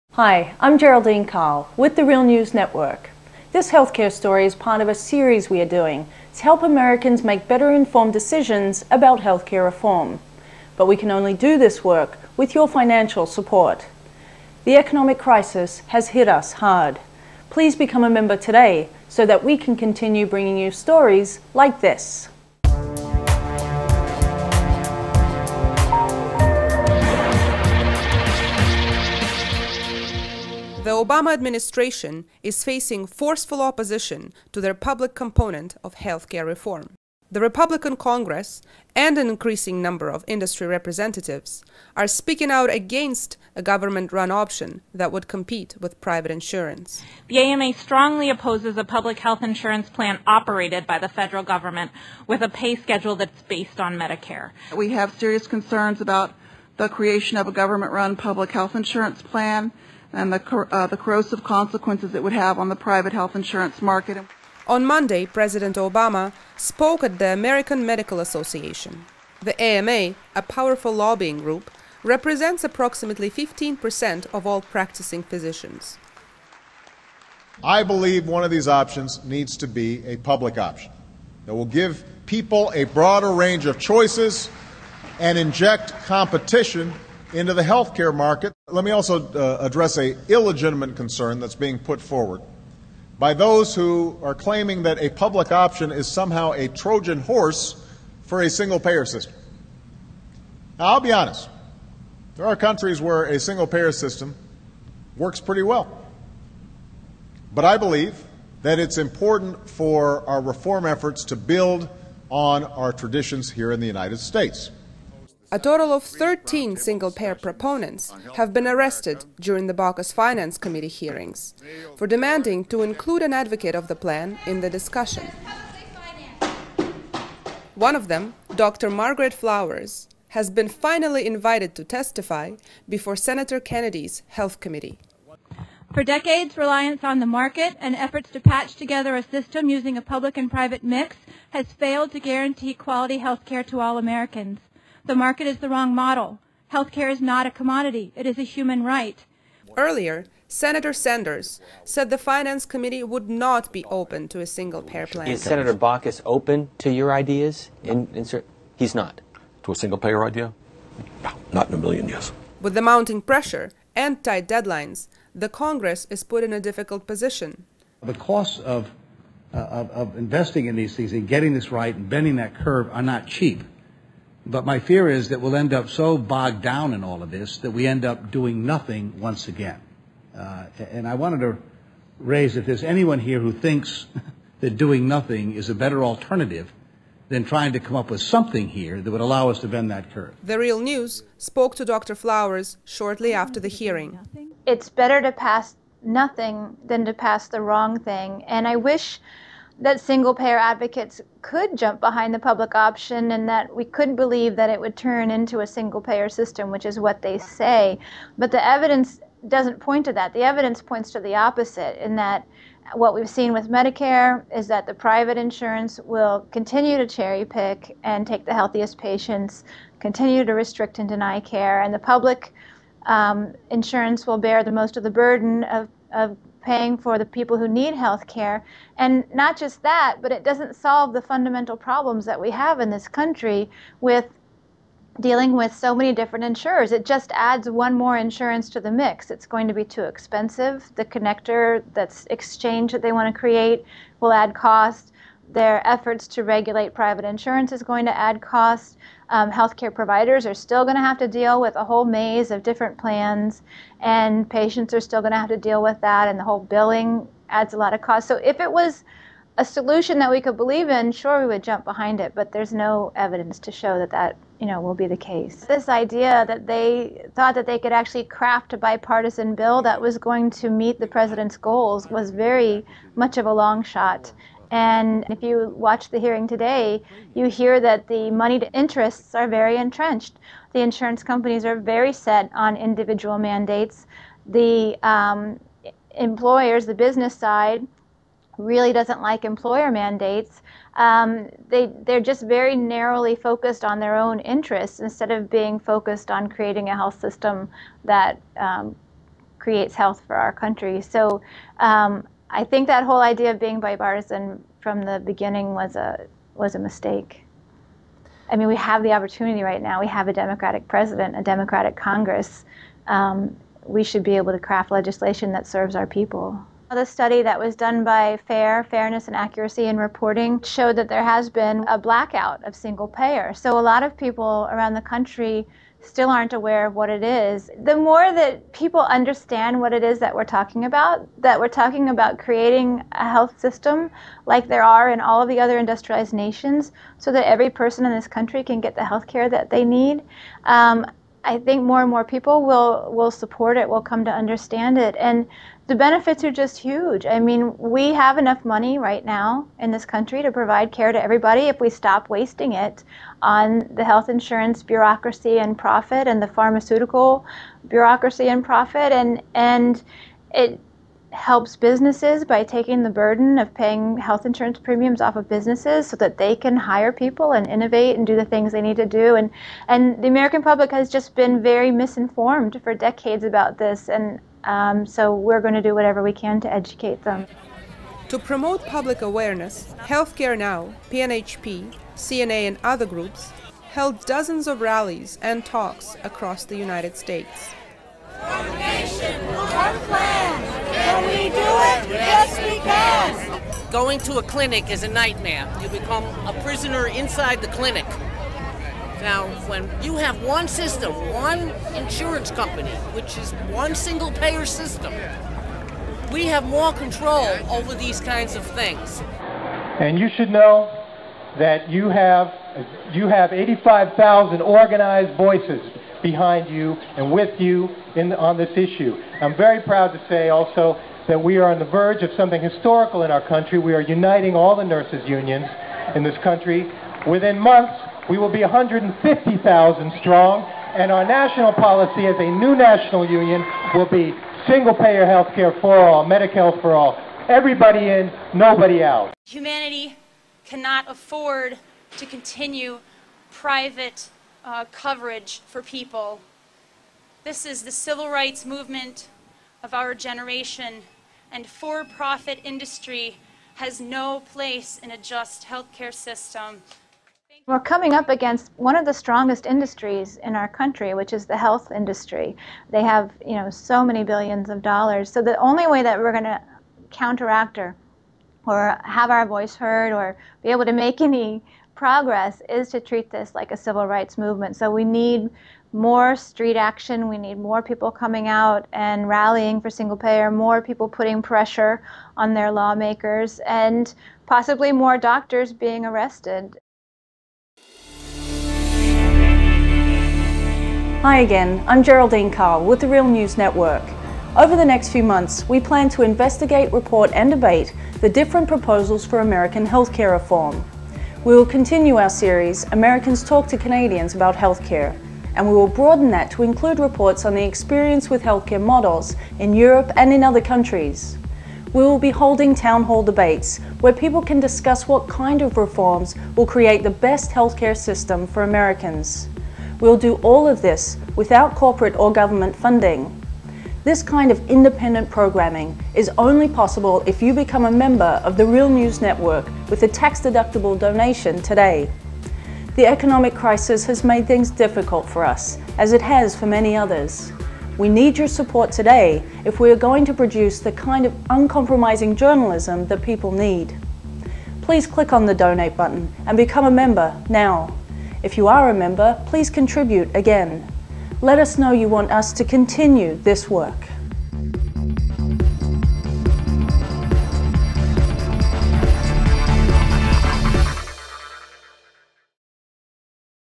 Health care a human right?
A single payer rep finally testifies to Congress; insurance industry steps up attack on any public plan